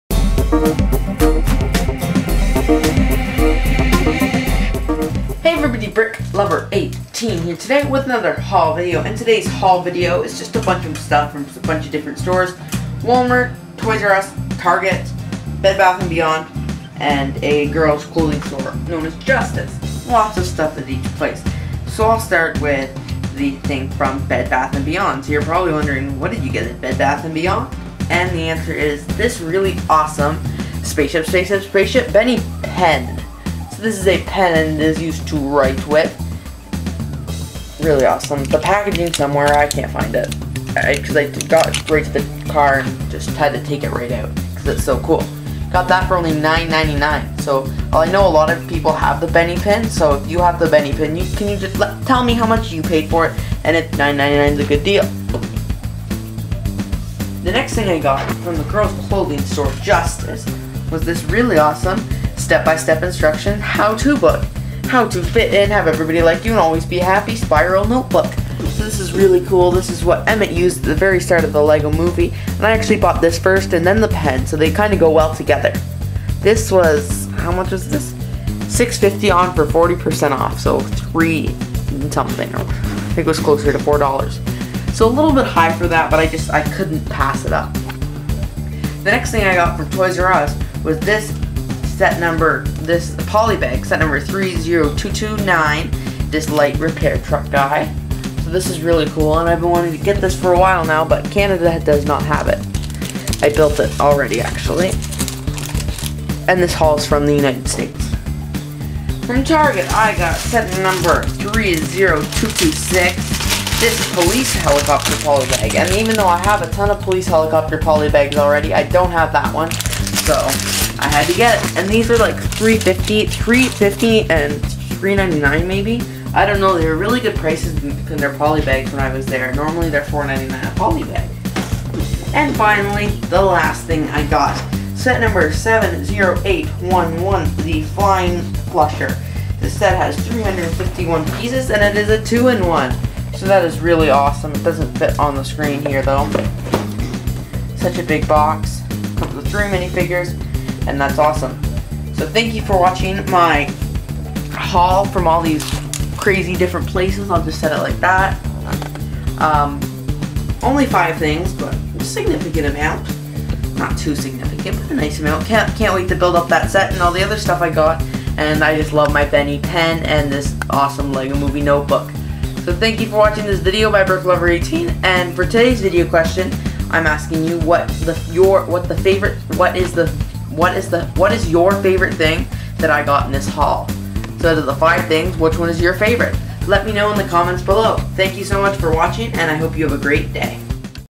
LEGO "TOYS R US", "WAL-MART" & "TARGET" LEGO HAUL
Today I have a LEGO haul from Toys R Us, Wal-Mart, Target and Bed Bath and Beyond.

Today's Video Question: How much did you pay for your Benny pen?
*please anwser in the comments!

A: Yes